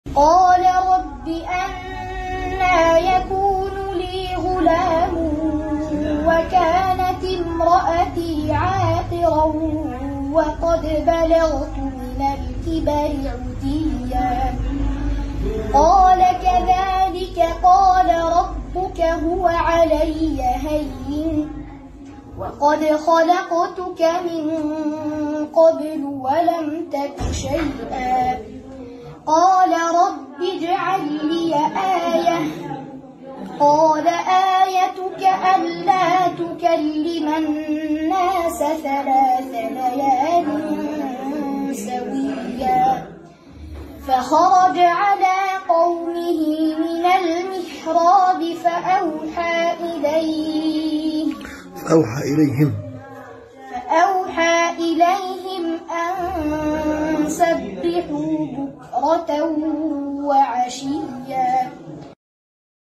🔹QS. Maryam: 8-11 ||🎙الطفل من دولة ليبيا ويقرأ بروآية قالون عن الإمام نافع
Makna Ayat✨
▪Zakaria berkata dengan penuh keheranan, ”wahai tuhanku, bagaimana bisa aku memiliki anak lelaki, sedang istriku adalah seorang wanita yang mandul yang tidak bisa melahirkan, dan aku sendiri telah mencapai batas akhir dalam usia dan tulangku pun sudah rapuh?
▪Malaikat menjawab Zakaria terhadap apa yang diherankannya, ”demikianlah keadaannya, sebagaimana yang engkau katakan, bahwa istrimu mandul dan engkau telah mencapai usia lanjut, akan tetapi tuhanmu telah berkata, bahwa penciptaan yahya dalam keadaan demikian ini merupakan perkara mudah lagi ringan bagi Ku. Kemudian Allah menyebutkan kepada zakaria perkara yang jauh lebih aneh daripada apa yang dipertanyakannya itu. Allah berfirman, ’Dan sesungguhnya aku telah menciptakanmu sebelum yahya, sedang kamu bukanlah sesuatu yang pernah disebut-sebut dan belum ada wujudnya sama sekali. ’
▪Zakaria berkata untuk menambah keyakinannya, ”wahai tuhanku, buatkanlah untukku tanda atas terwujudnya kabar gembira yang telah disampaikan malaikat kepadaku,” Allah berfirman ”tandamu, bahwa engkau tidak bisa berbicara kepada manusia selama tiga hari tiga malam, padahal engaku sehat-sehat saja dan bebas dari penyakit.
▪Lalu Zakaria keluar dari tempat ibadahnya menemui kaumnya, yaitu dari tempat disampaikannya kabar gembira kepadanya untuk meemperoleh anak. Dia memberikan isyarat kepada mereka, ”hendaklah kalian bertasbih menyucikan Allah pada pagi dan sore sebagai bentuk ungkapan syukur kepada Allah . ”

🔹QS. Maryam: 8-11
🎙الطفل من دولة ليبيا ويقرأ بروآية قالون عن الإمام نافع
📖Tafsir Al-Muyassar